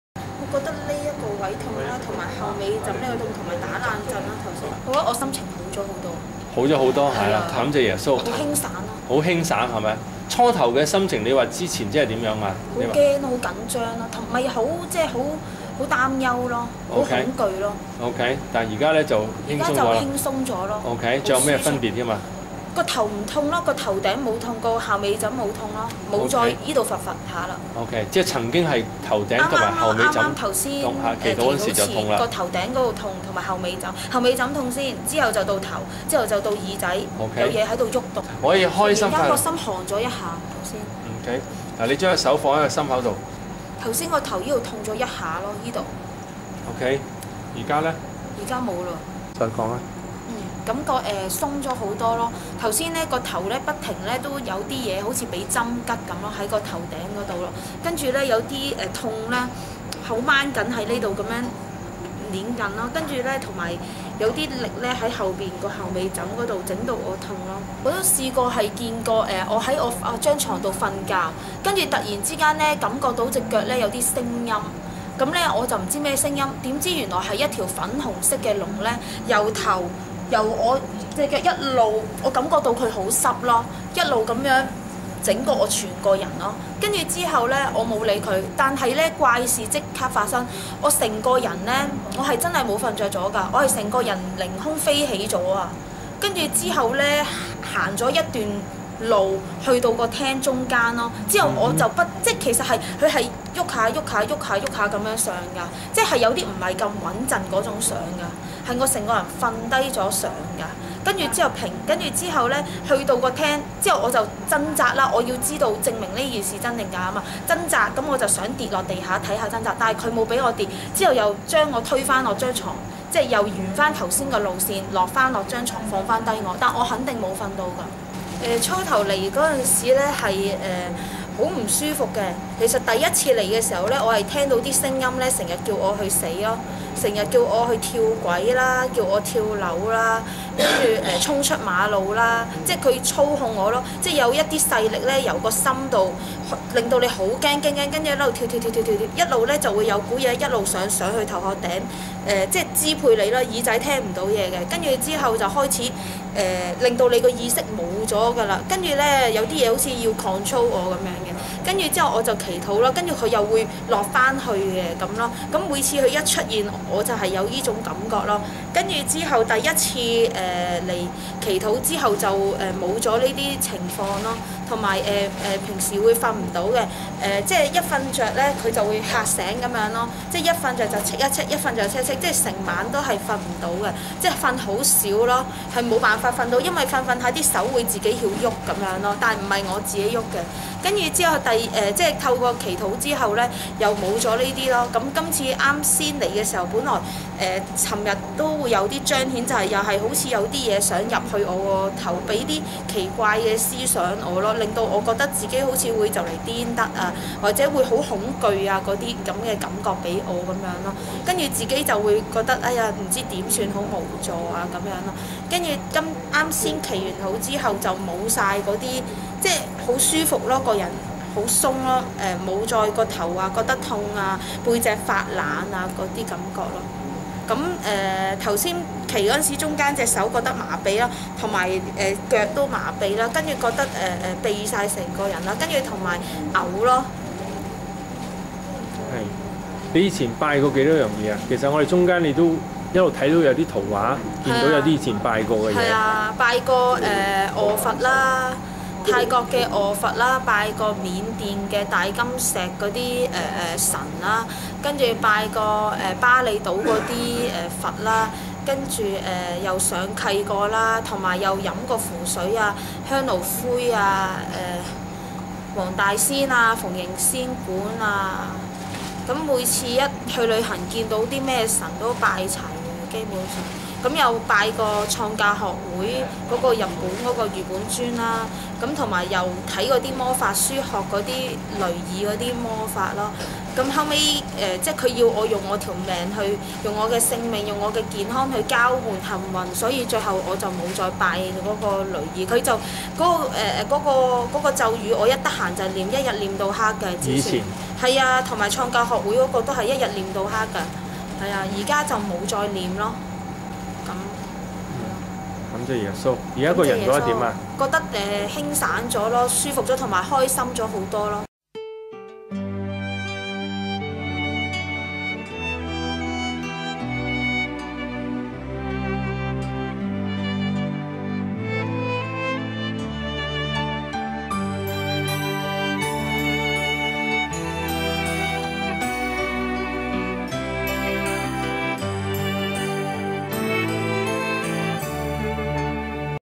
祈禱中釋放泰國，黃大仙，印尼，符咒等邪靈 葉牧師
祈禱中釋放泰國，黃大仙，印尼，符咒等邪靈
1．星期日：3:15崇拜；2:30服事禱告；崇拜後有小組及侍奉訓練課程。
2．星期一：4時服事禱告，7時侍奉訓練課程。